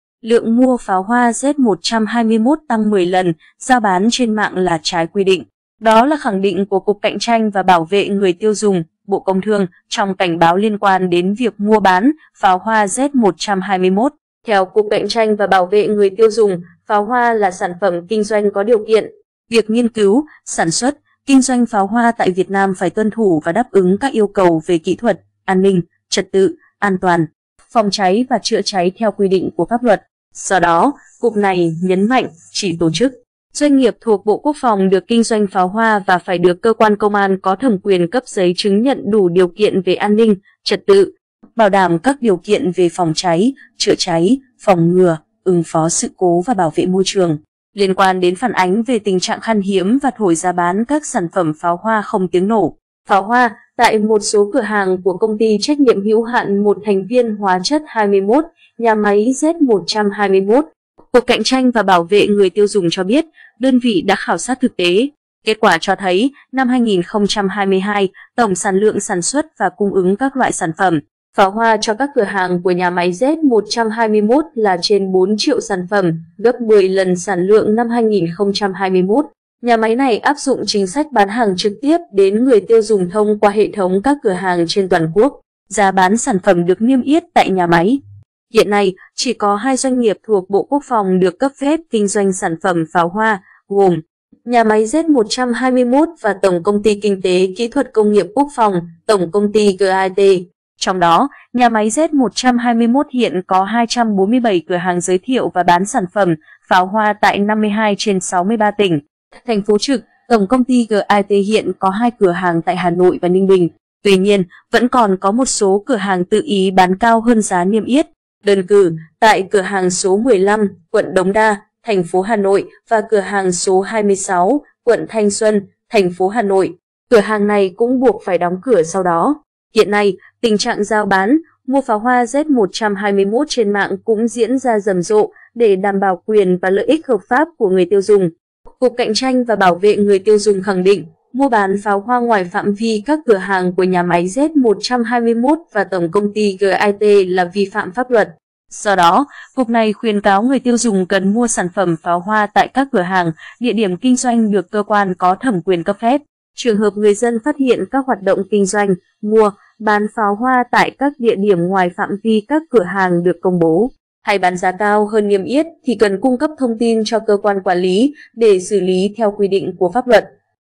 Lượng mua pháo hoa Z121 tăng 10 lần, rao bán trên mạng là trái quy định
Đó là khẳng định của Cục Cạnh tranh và Bảo vệ người tiêu dùng (Bộ Công thương) trong cảnh báo liên quan đến việc mua bán pháo hoa Z121.Hiện nay, tình trạng rao bán, mua pháo hoa Z121 trên mạng cũng diễn ra rầm rộ, để đảm bảo quyền và lợi ích hợp pháp của người tiêu dùng, Cục Cạnh tranh và Bảo vệ người tiêu dùng khẳng định, mua bán pháo hoa ngoài phạm vi các cửa hàng của Nhà máy Z121 và Tổng công ty GAET là vi phạm pháp luật.
Kết quả cho thấy, năm 2022, tổng sản lượng sản xuất và cung ứng các loại sản phẩm pháo hoa cho các cửa hàng của Nhà máy Z121 là trên 4 triệu sản phẩm, gấp 10 lần sản lượng năm 2021.
Theo Cục Cạnh tranh và Bảo vệ người tiêu dùng, pháo hoa là sản phẩm kinh doanh có điều kiện. Việc nghiên cứu, sản xuất, kinh doanh pháo hoa tại Việt Nam phải tuân thủ và đáp ứng các yêu cầu về kỹ thuật, an ninh, trật tự, an toàn, phòng cháy và chữa cháy theo quy định của pháp luật.
Tổng sản lượng pháo hoa năm 2022 trên 4 triệu sản phẩm, gấp 10 lần sản lượng năm 2021
Trường hợp người dân phát hiện các hoạt động kinh doanh, mua, bán pháo hoa tại các địa điểm ngoài phạm vi các cửa hàng được công bố, hay bán giá cao hơn niêm yết thì cần cung cấp thông tin cho cơ quan quản lý, để xử lý theo quy định của pháp luật.
Liên quan đến phản ánh về tình trạng “khan hiếm” và “thổi giá bán” các sản phẩm pháo hoa không tiếng nổ (pháo hoa) tại một số cửa hàng của Công ty TNHH MTV Hóa chất 21 (Nhà máy Z121), Cục Cạnh tranh và Bảo vệ người tiêu dùng cho biết, đơn vị đã khảo sát thực tế.
Do đó, Cục này nhấn mạnh: "Chỉ tổ chức, doanh nghiệp thuộc Bộ Quốc phòng được kinh doanh pháo hoa và phải được cơ quan Công an có thẩm quyền cấp giấy chứng nhận đủ điều kiện về an ninh, trật tự; bảo đảm các điều kiện về phòng cháy, chữa cháy, phòng ngừa, ứng phó sự cố và bảo vệ môi trường".
Tuy nhiên, vẫn còn có một số cửa hàng tự ý bán cao hơn giá niêm yết. Đơn cử, tại cửa hàng số 15 (quận Đống Đa, TP Hà Nội) và cửa hàng số 26 (quận Thanh Xuân, TP Hà Nội). Cửa hàng này cũng buộc phải đóng cửa sau đó.
Nhà máy này áp dụng chính sách bán hàng trực tiếp đến người tiêu dùng thông qua hệ thống các cửa hàng trên toàn quốc. Giá bán sản phẩm được niêm yết tại nhà máy.
Hiện nay, chỉ có 2 doanh nghiệp thuộc Bộ Quốc phòng được cấp phép kinh doanh sản phẩm pháo hoa, gồm: Nhà máy Z121 và Tổng Công ty Kinh tế kỹ thuật Công nghiệp quốc phòng (Tổng công ty GAET). Trong đó, Nhà máy Z121 hiện có 247 cửa hàng giới thiệu và bán sản phẩm pháo hoa tại 52/63 tỉnh, thành phố trực; Tổng công ty GAET hiện có 2 cửa hàng tại Hà Nội và Ninh Bình.
Do đó, Cục này khuyến cáo người tiêu dùng cần mua sản phẩm pháo hoa tại các cửa hàng, địa điểm kinh doanh được cơ quan có thẩm quyền cấp phép.